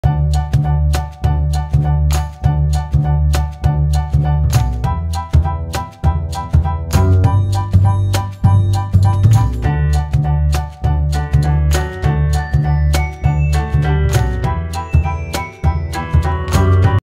Dog fights with his reflection in the Mirror (4K)
Here is a cute dog that sees it's reflection for the first time !

Fax Routine